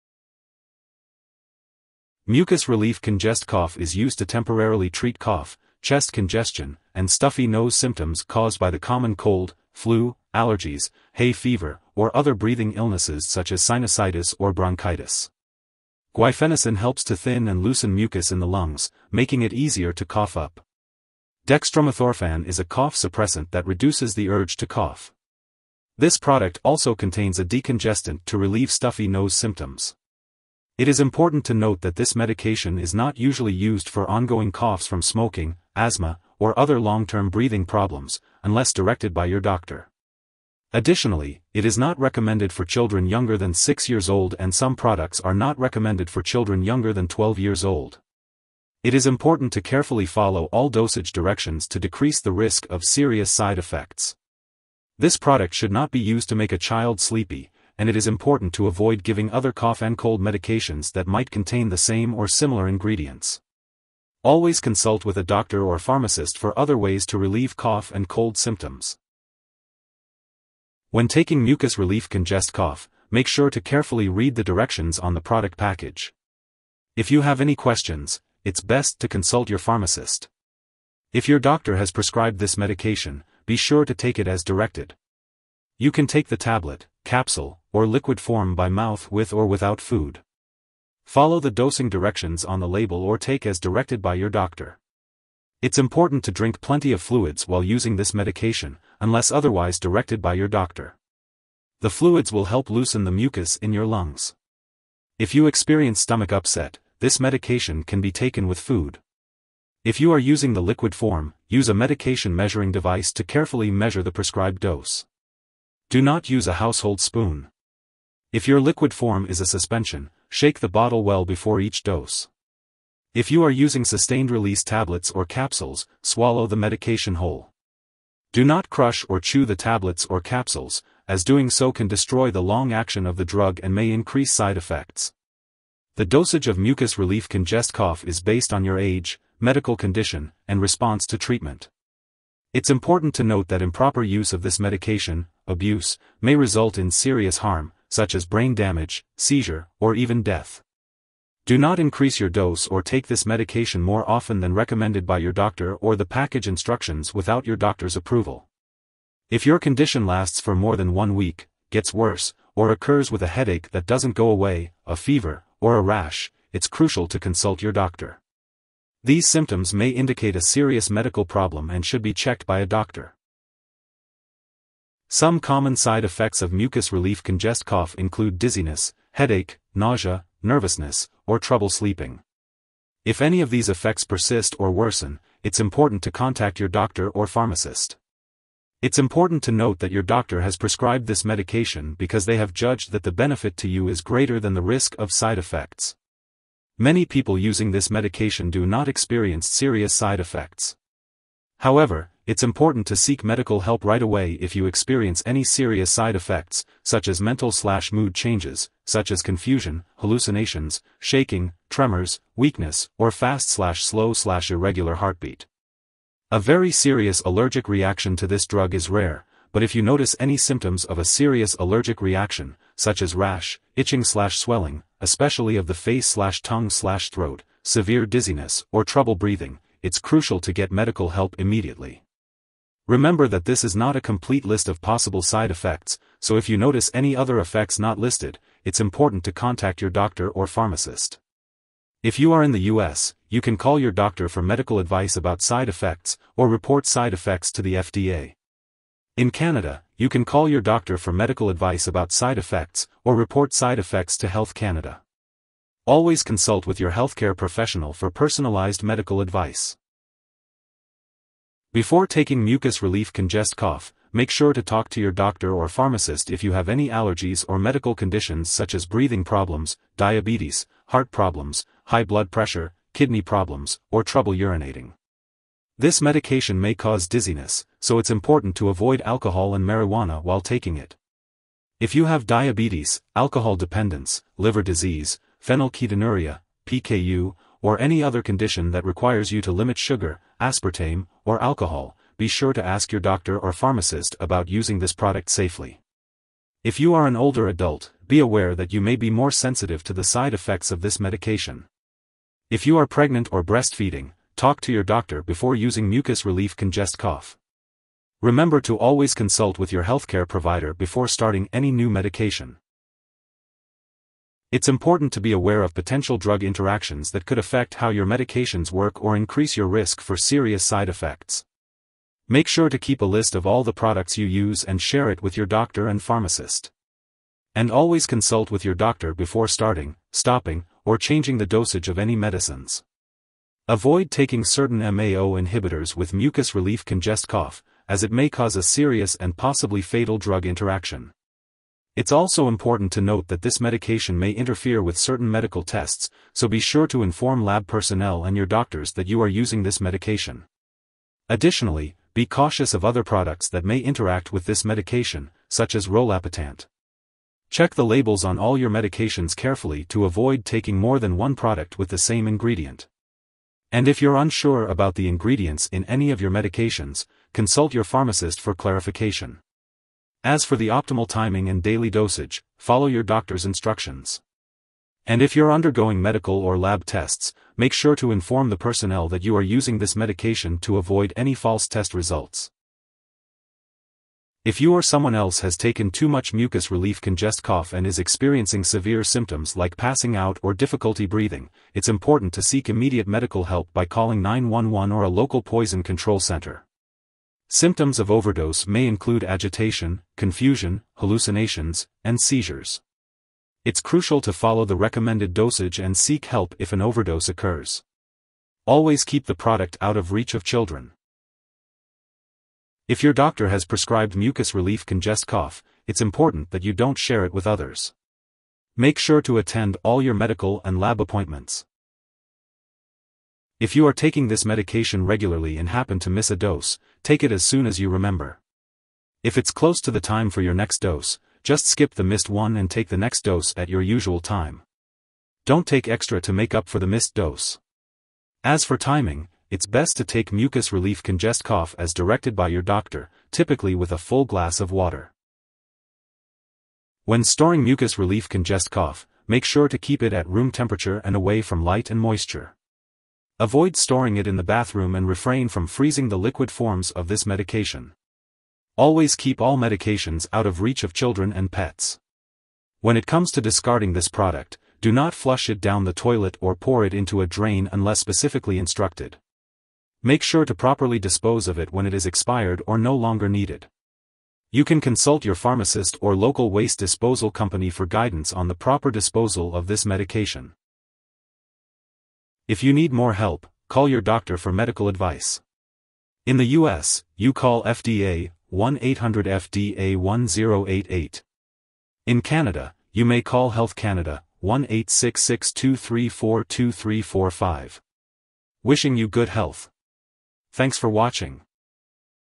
How to use Mucus Relief Congest-Cough - Explain Uses,Side Effects,Interactions
Mucus Relief Congest-Cough is used to temporarily treat cough, chest congestion, and stuffy nose symptoms caused by the common cold, flu, allergies, hay fever, or other breathing illnesses such as sinusitis or bronchitis. Guaifenesin helps to thin and loosen mucus in the lungs, making it easier to cough up. Dextromethorphan is a cough suppressant that reduces the urge to cough. This product also contains a decongestant to relieve stuffy nose symptoms. It is important to note that this medication is not usually used for ongoing coughs from smoking, asthma, or other long-term breathing problems, unless directed by your doctor. Additionally, it is not recommended for children younger than 6 years old and some products are not recommended for children younger than 12 years old. It is important to carefully follow all dosage directions to decrease the risk of serious side effects. This product should not be used to make a child sleepy, and it is important to avoid giving other cough-and-cold medications that might contain the same or similar ingredients. Always consult with a doctor or pharmacist for other ways to relieve cough and cold symptoms.
8701